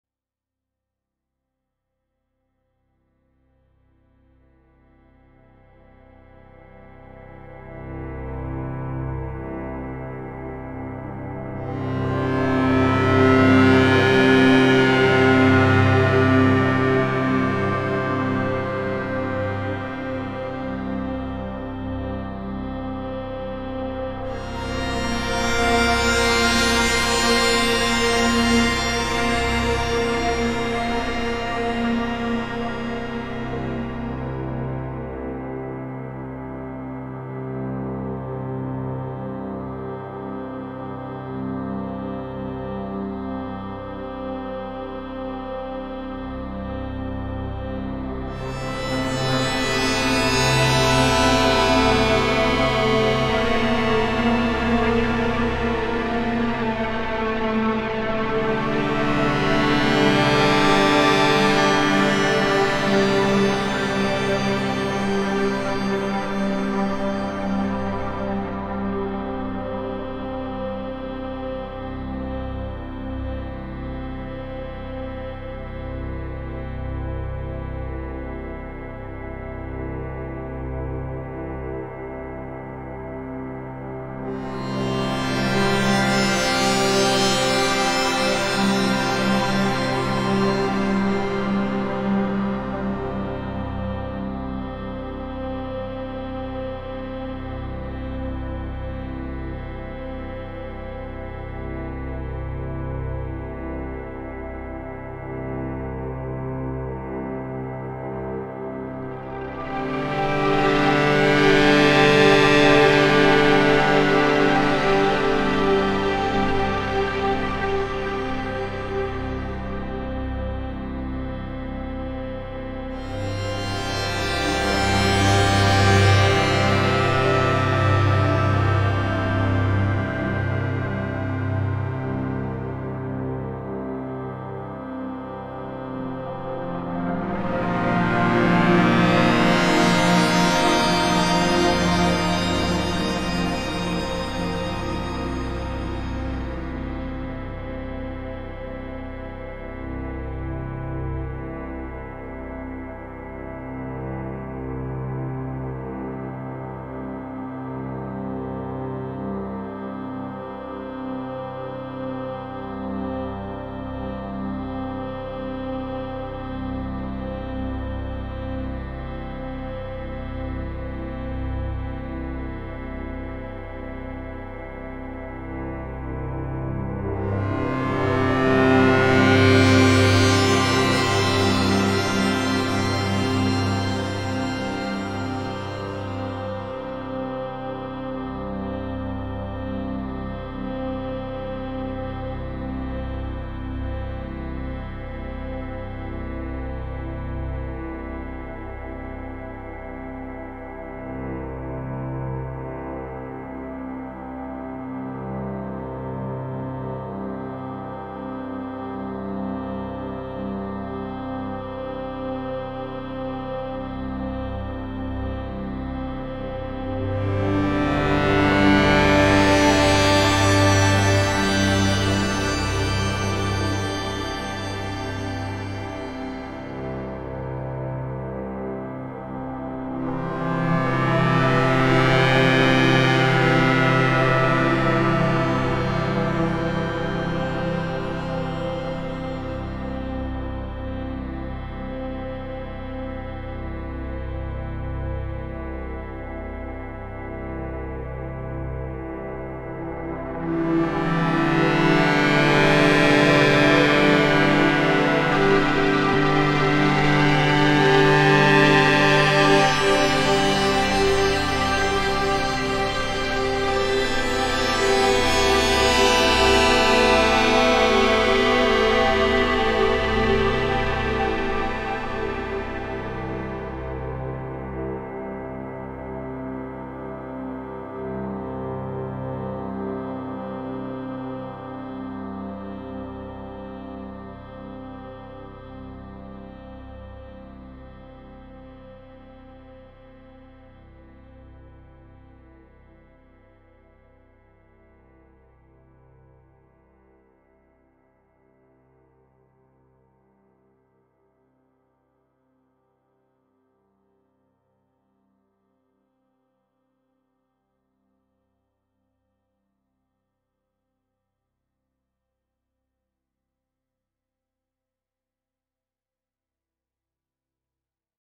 Ebb ensemble
First melodic runout for the 4ms Ensemble Oscillator which is providing an ebb and flow texture, following a grainy lead voice from Plaits. 4ms SWN provides the backdrop. drives via Shuttle Control